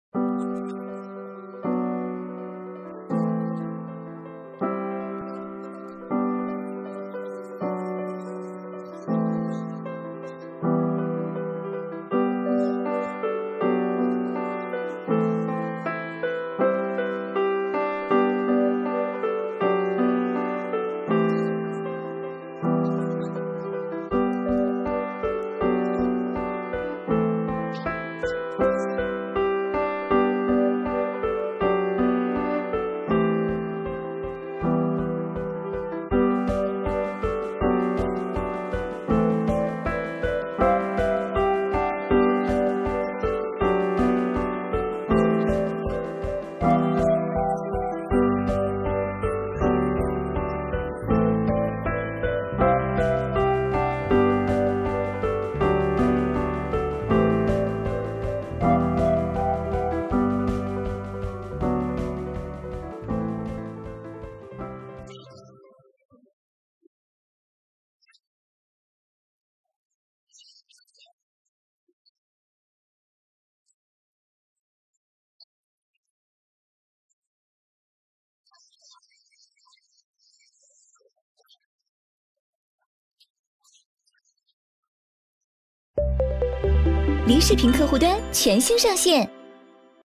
影后不是盖的！赵丽蓉-20℃拒替身
1928年3月11日，赵丽蓉出生在天津。除了著名评剧、小品表演艺术家的身份，她还是影后——26年前凭借《过年》荣获东京国际电影节最佳女演员。